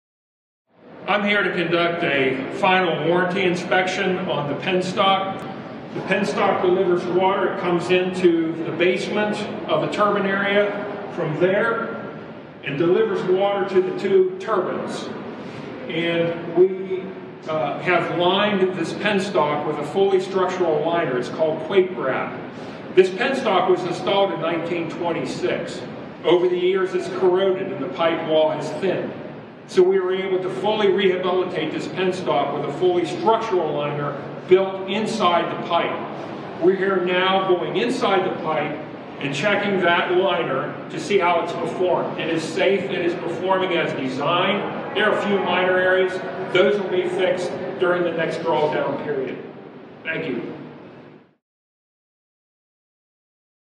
Penstock Inspection with Kurt Wright
Kurt Wright, PE, SDG Engineering Inc. explains the Lake Lure penstock inspection.